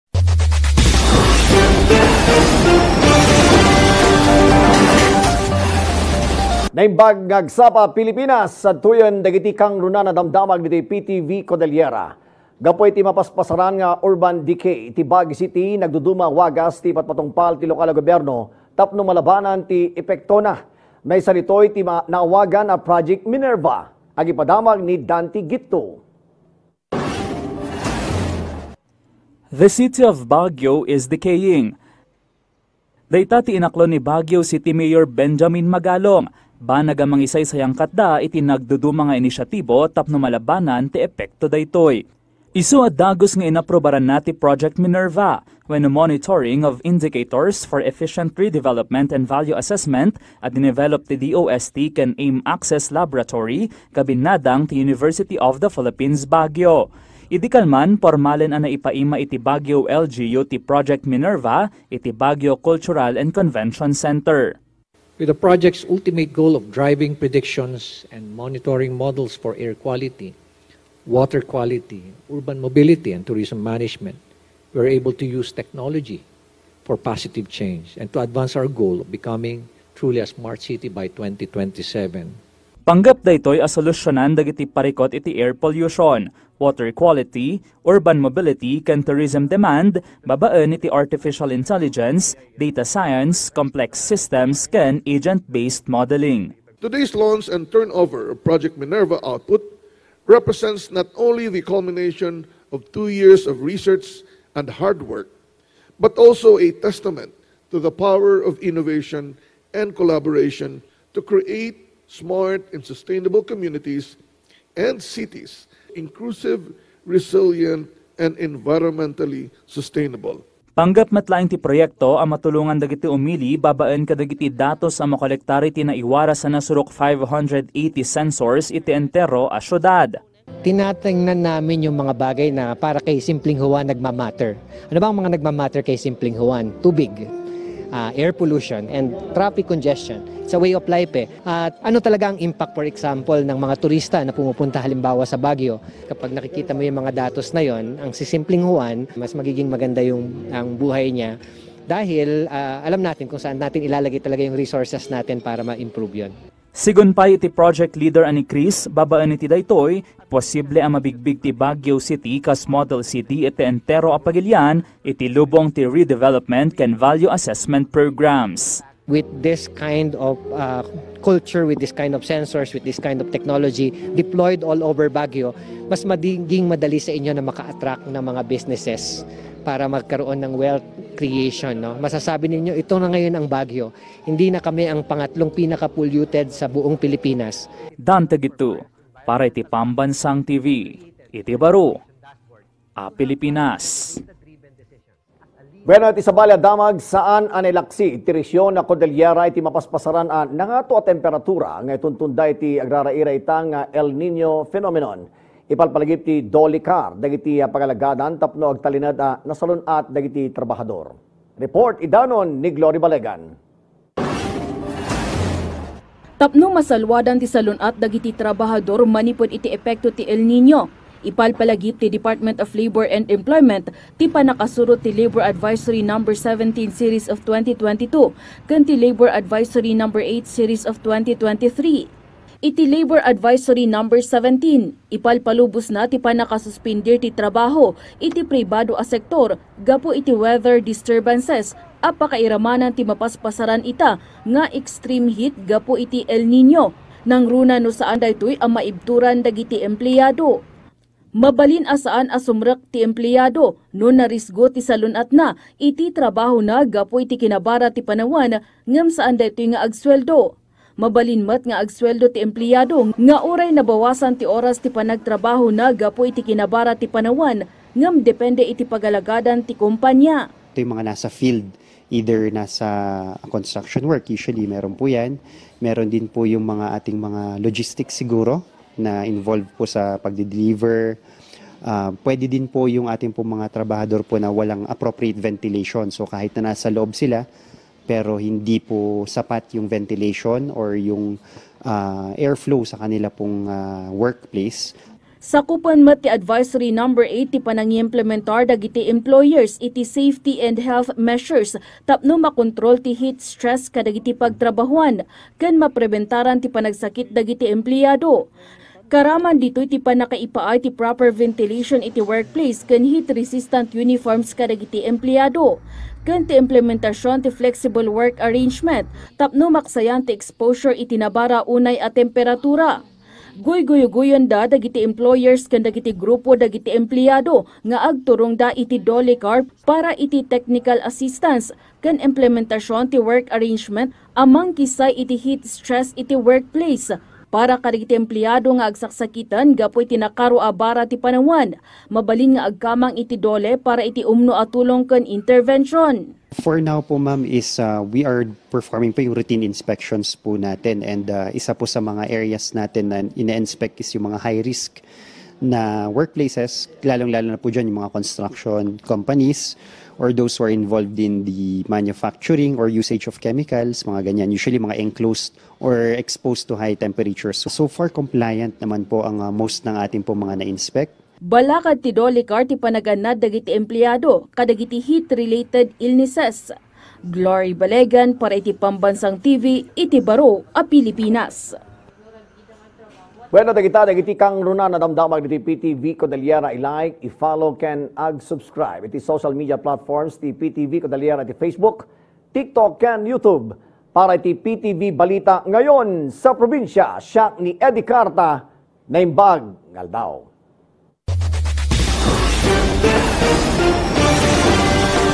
Project 'Minerva', napasakamay ng Baguio City
Work arrangement ngayong mainit ang panahon, ipinaalala ng DOLE-CAR